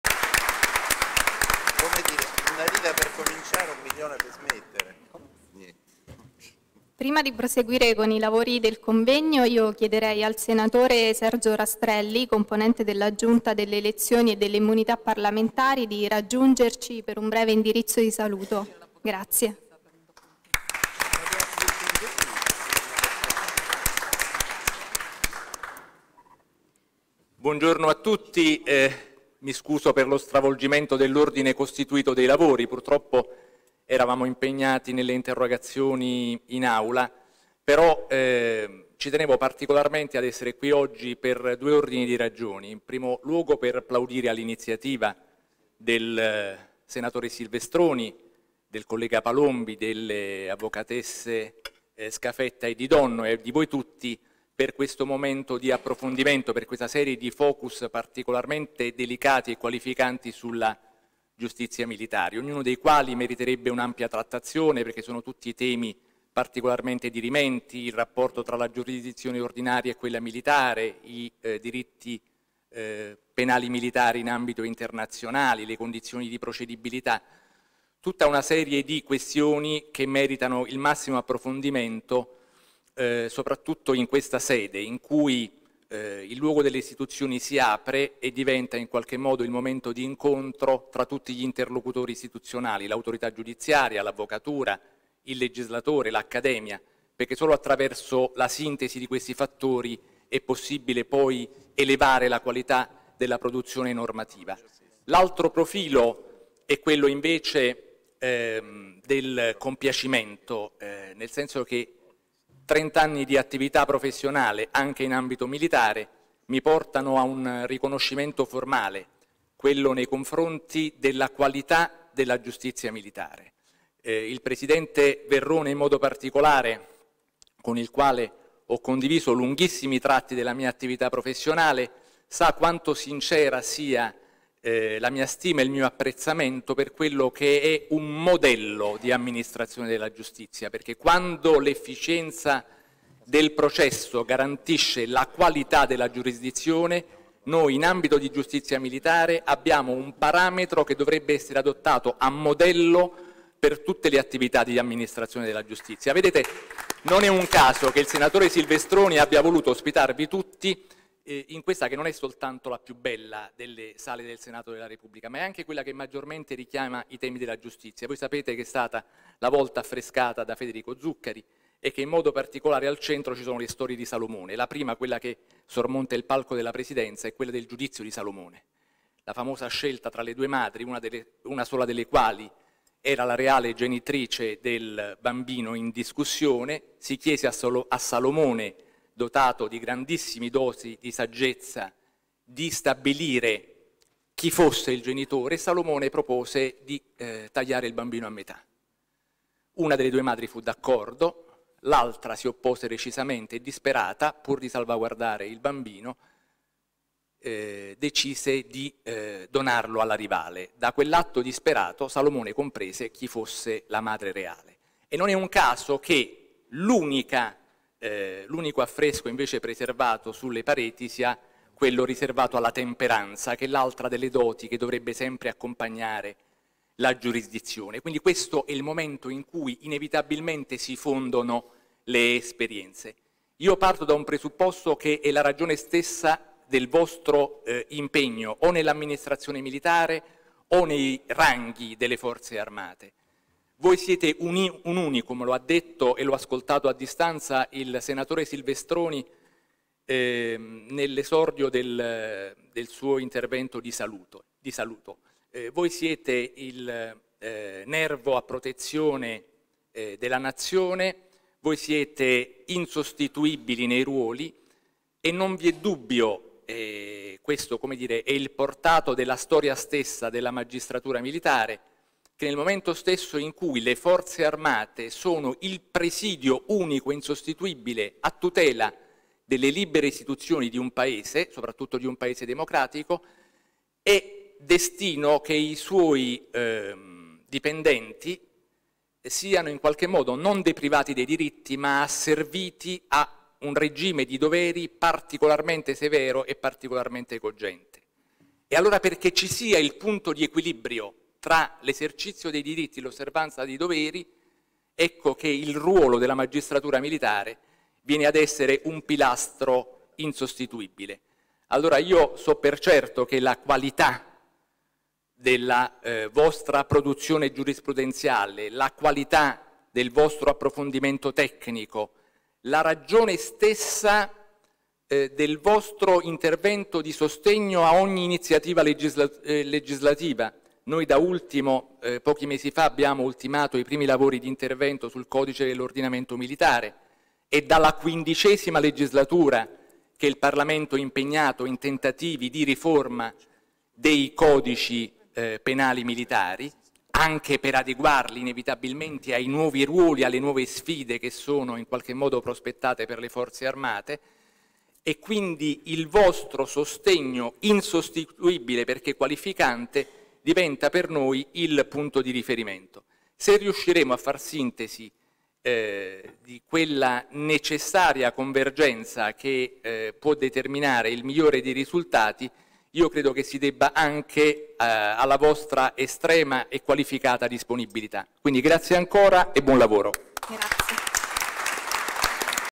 Diritto penale militare - Sergio Rastrelli
Sergio Rastrelli, Senatore componente della Giunta delle elezioni e delle immunità parlamentari, saluta il pubblico al convegno presso il Senato sul diritto penale militare.